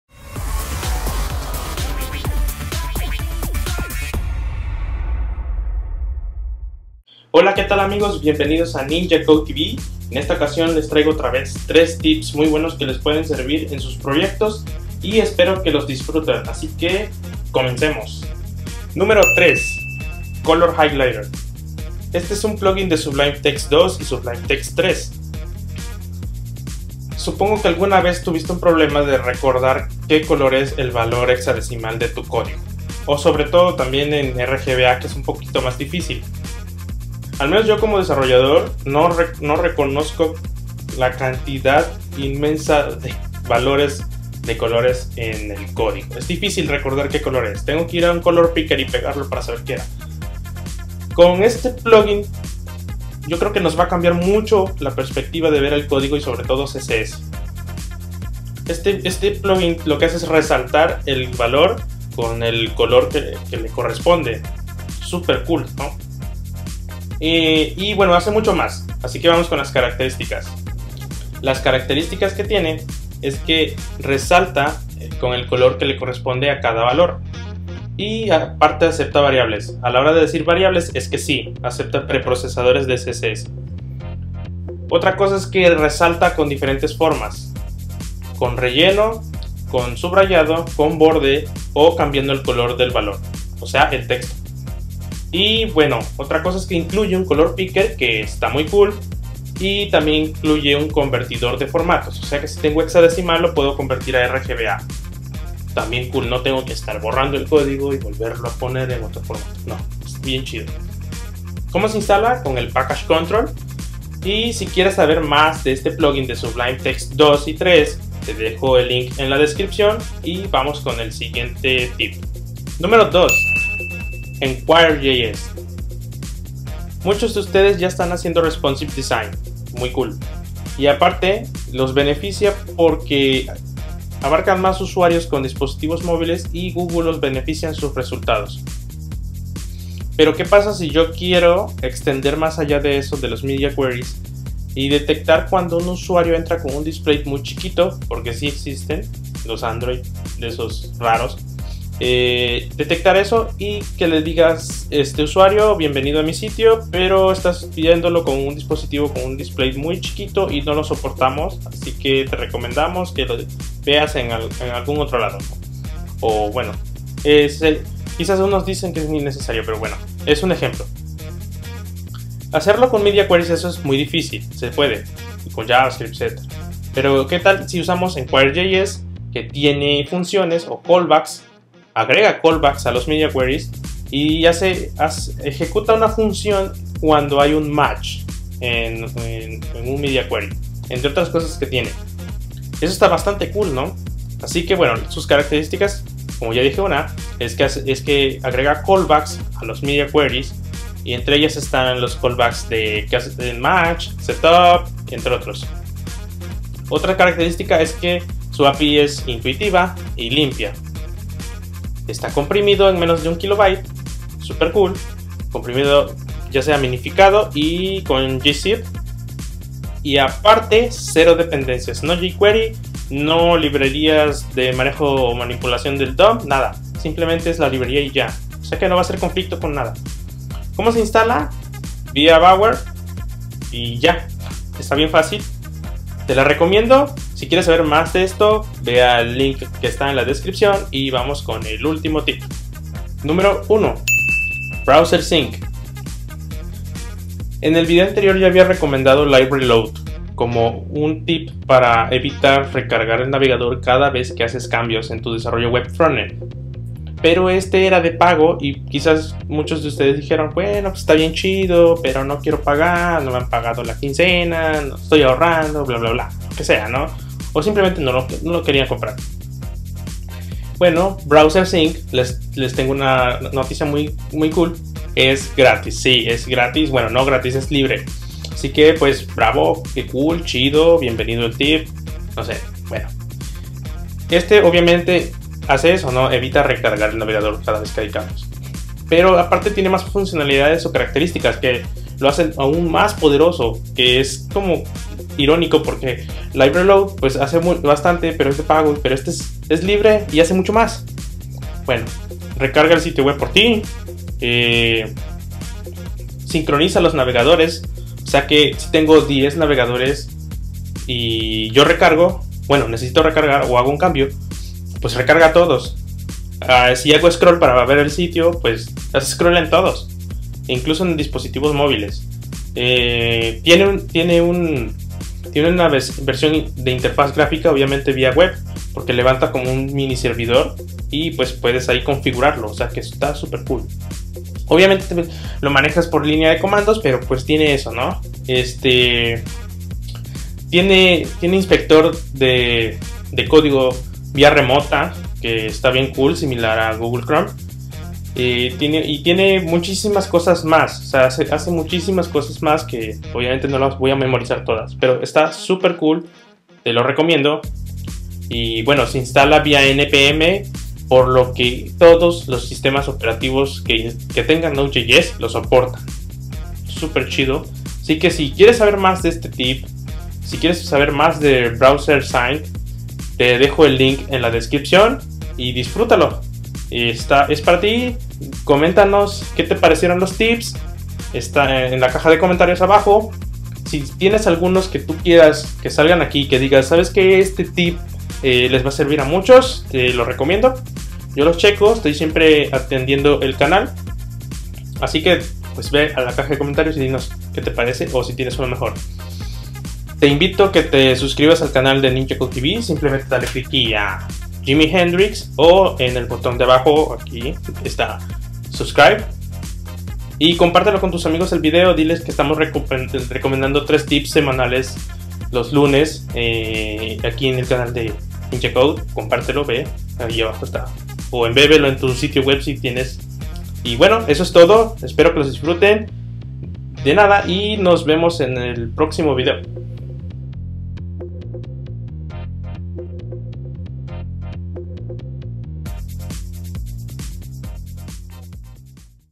Tips #6 - Color Highlighter, Enquire.js y Browsersync
Esta vez te recomendamos estos tips para mejorar la calidad y tiempo de desarrollo en tus proyectos.

1 - Browsersync: Herramienta para evitar recargar sitios en desarrollo y mucho más.